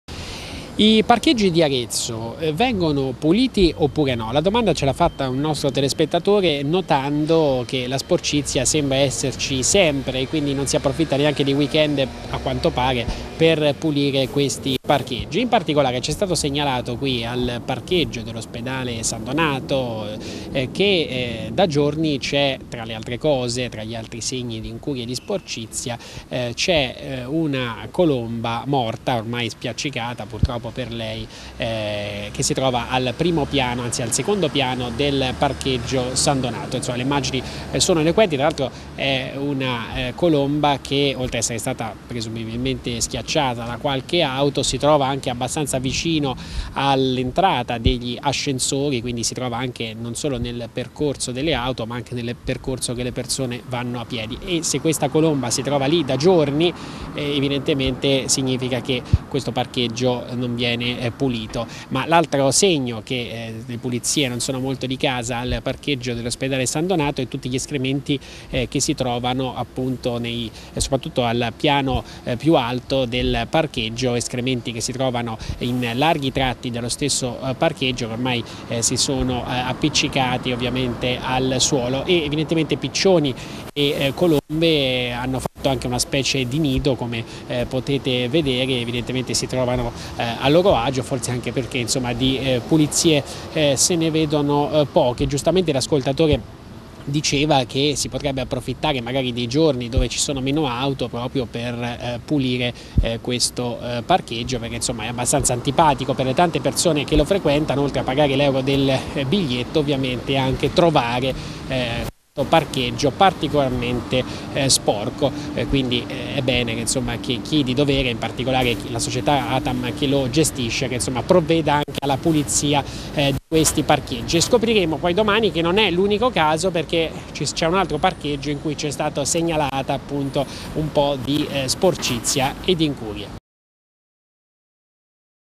Arezzo, sporcizia e scarsa pulizia al parcheggio dell'ospedale
Un telespettatore ci ha segnalato la situazione di scarsa polizia del parcheggio coperto che conduce all'ospedale San Donato. Colombe morte per terra, escrementi ed altri tipi di rifiuti. Servizio andato in onda su ArezzoTv il 07/07/2015